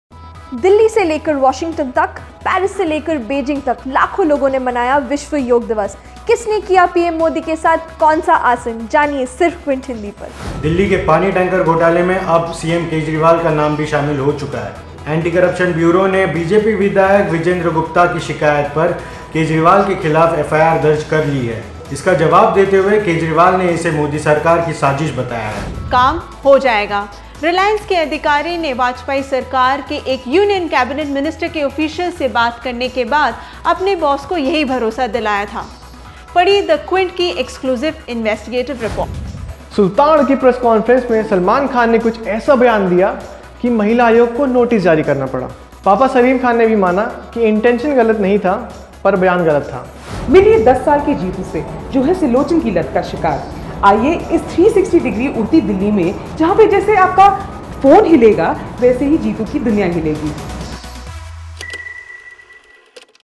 Q एक्सप्रेस: सलमान के खिलाफ नोटिस जारी, एस्सार टेप्स, उड़ती दिल्ली
दिन की सबसे बड़ी और खास खबरें बस एक मिनट में